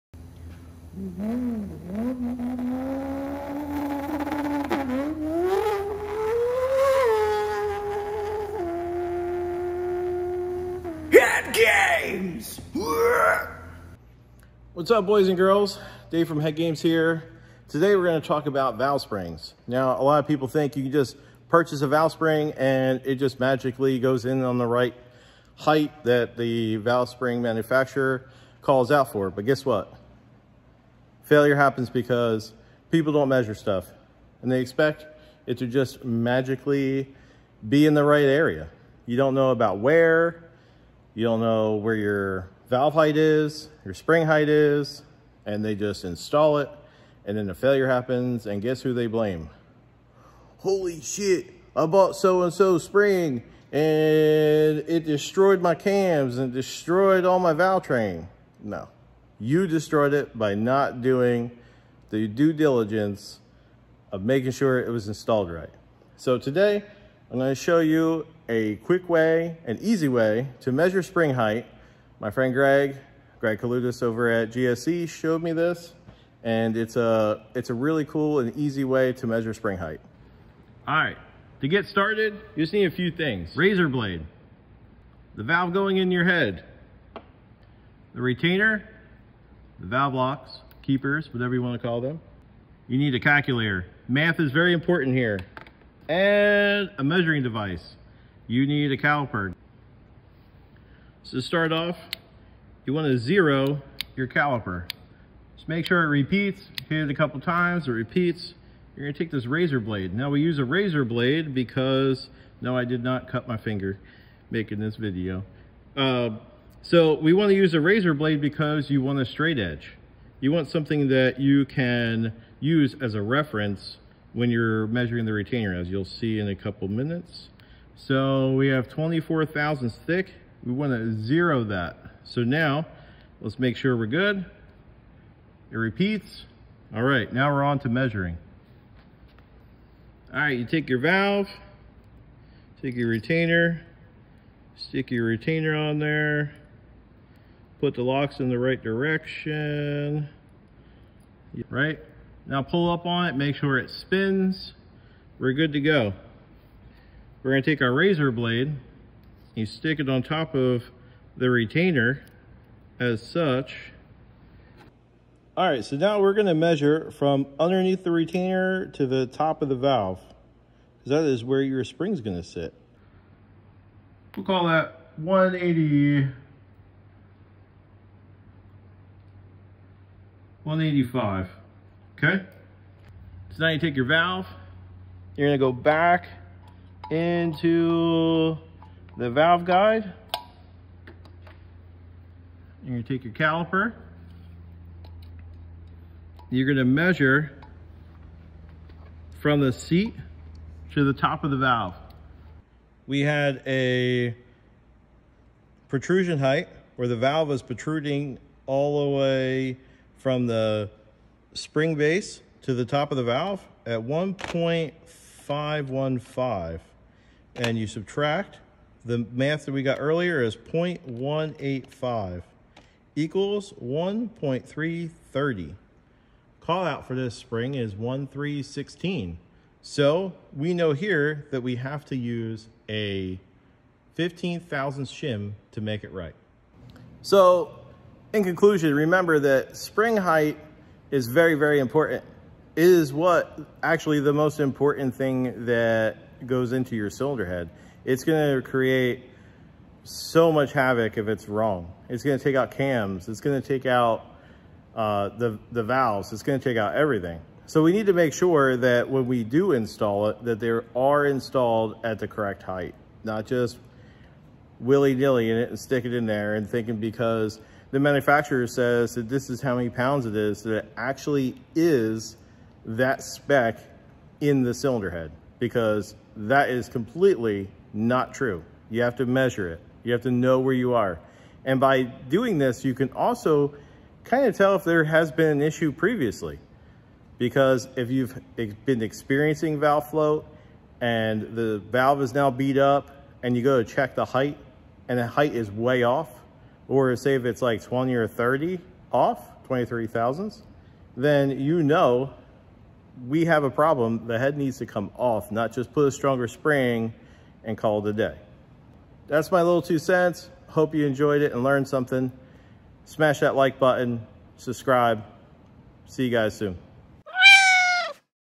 Checking spring height on a 2JZ!
For the novice guy or gal, when doing spring height on a 2JZ, it’s hard to measure when there are no tools available to accomplish this.. So, here is a way that anyone can do it! The bonus — this method actually works on more heads than just the 2JZ! Almost any head can be measured by doing this method.
Now, you WON’T be able to do this with the head on the car, (well you could but it would take some more measurements and ingenuity)  but if you have the head off and disassembled and ready to not just plop a spring in the head. This will only take a few minutes and ensure that it’s good to go!

tools needed to check spring height this way
—Caliper (yes, the Harbor Freight calipers work great!)
— Calculator
— note pad and pen
— razor blade